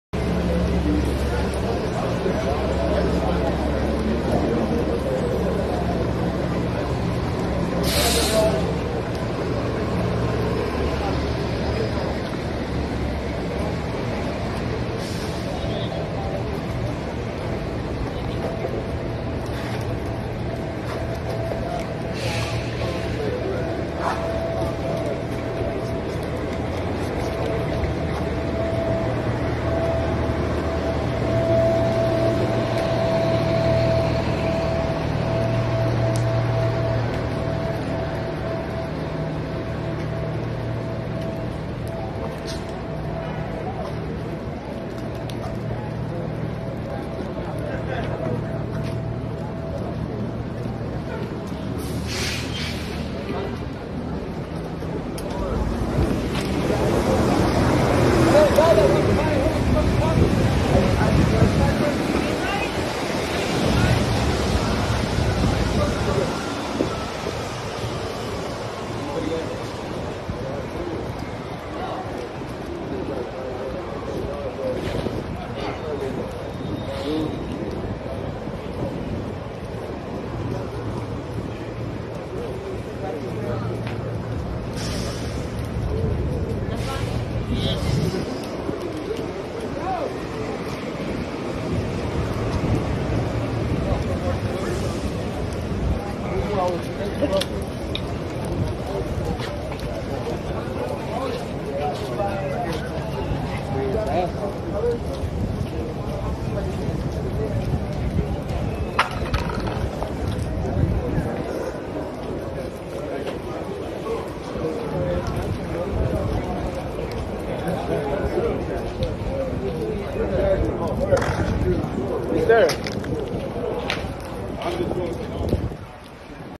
Сиэтл (США) утопает в бездомных и наркоманах...
Сиэтл (США) утопает в бездомных и наркоманах... Городской совет Сиэтла отклонил законопроект о судебном преследовании за употребление и хранение наркотиков на открытом воздухе.